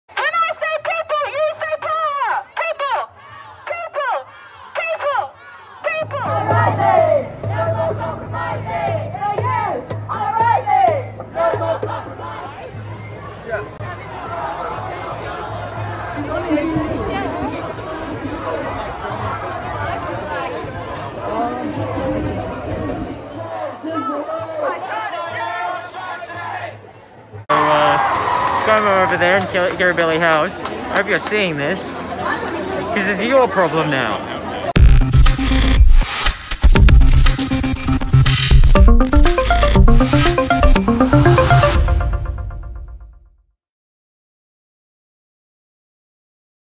The youth are rising!
"The youth are rising! No more compromising!" – this was one of the chants at the Sydney school climate strike on March 25.  Young people had a strong message for Australian Prime Minister Scott Morrison. Video shot by Peter Boyle on Gadigal land. Always was, always will be, Aboriginal land.